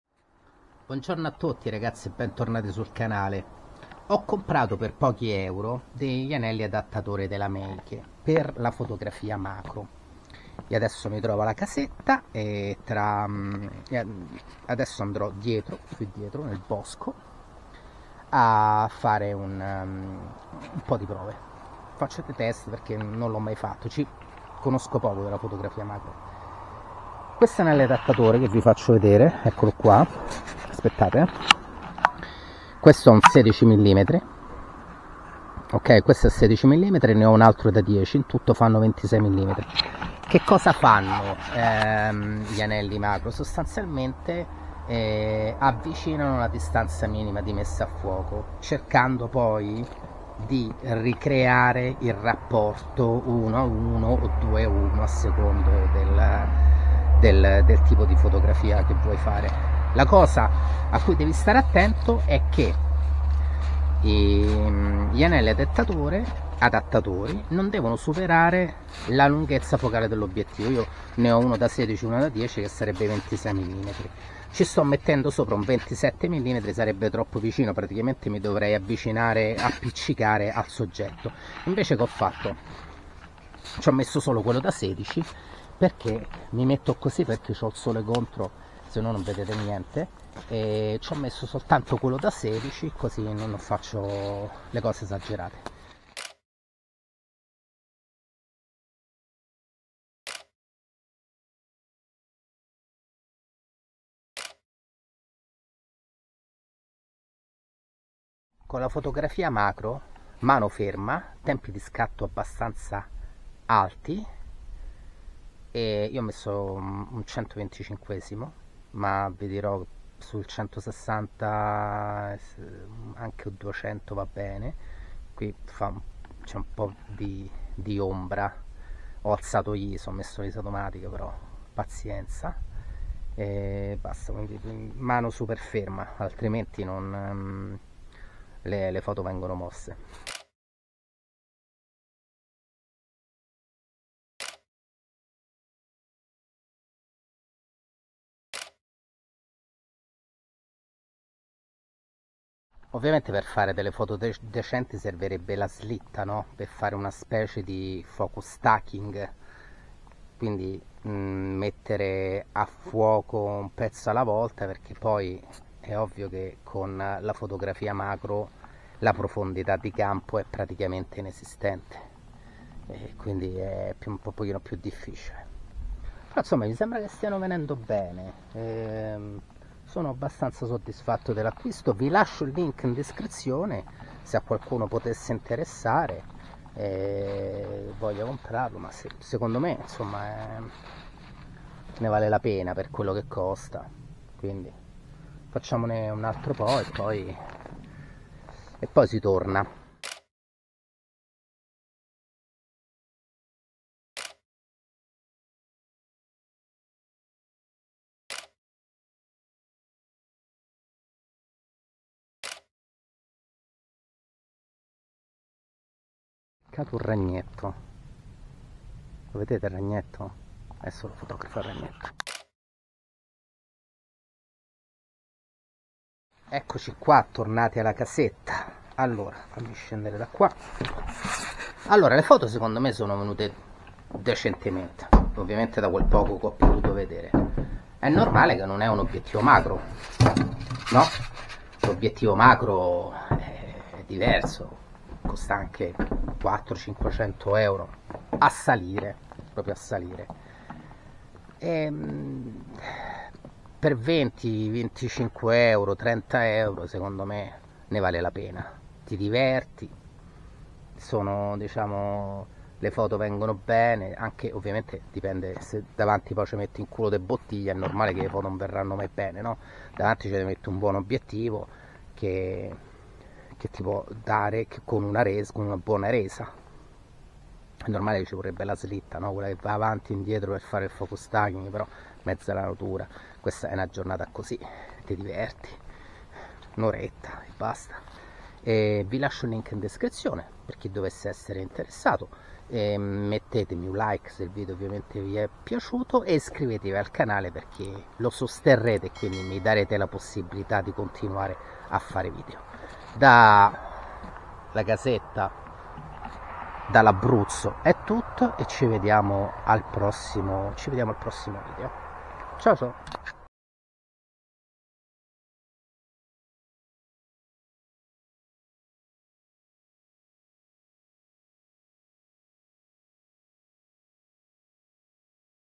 Foto MACRO a 30 Euro. (Mini Video)
Con circa 30 Euro ho acquistato i tubi di prolunga della Meike per sperimentare la magia della fotografia MACRO. E il risultato? Semplicemente incredibile! Ho ottenuto immagini molto belle senza dover spendere una fortuna. E non è tutto: questi tubi si rivelano anche ottimi per la scansione delle pellicole. Curiosi di vedere i risultati? Date un'occhiata a questo breve video!"